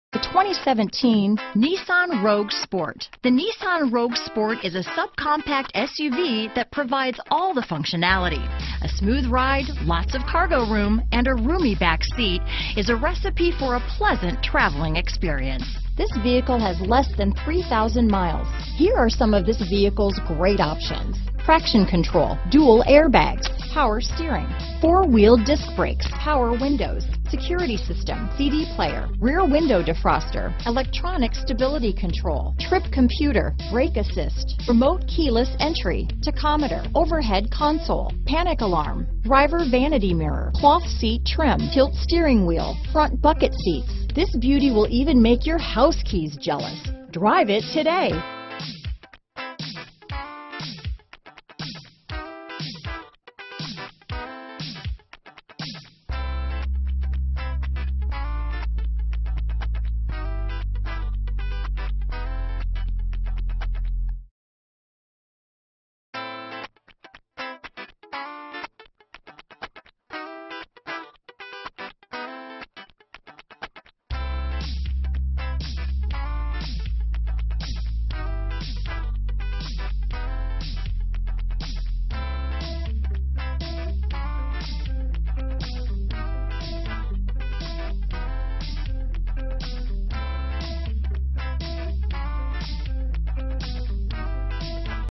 2017 Nissan Rogue Sport Niles, Skokie, Chicago, Evanston, Park Ridge, IL R26911
U 2017 Nissan Rogue Sport

Star Nissan
5757 W Touhy Ave

Clean CARFAX. Certified. White 2017 Nissan Rogue Sport S FWD CVT with Xtronic 2.0L DOHC Nissan Certified Pre-Owned Certified.brbrOdometer is 930 miles below market average! 32/25 Highway/City MPGbrbrNissan Certified Pre-Owned Details:brbr  * 167 Point Inspectionbr  * Vehicle Historybr  * Roadside Assistancebr  * Powertrain Limited Warranty: 84 Month/100,000 Mile (whichever comes first) from original in-service datebr  * Warranty Deductible: $50br  * Includes Car Rental and Trip Interruption Reimbursement